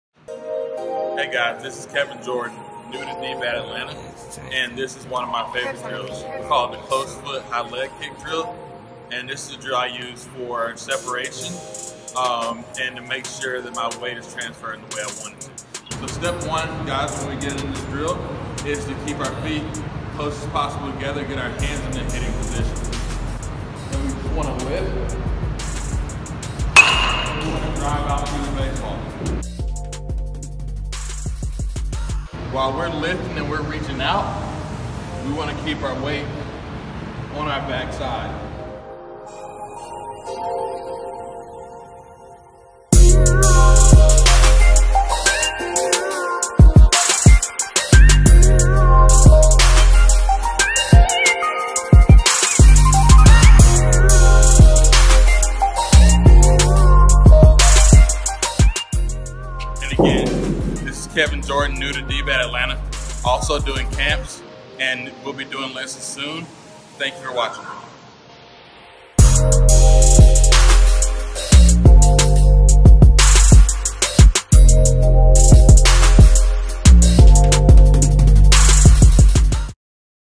Kevin Key Tips. Volume1
Kevin shows us the drill he uses for when he struggles transferring weight from the back side of the lower half, to the front and through the ball with power. The drill is exaggerated to train the body to transfer and stay on balance.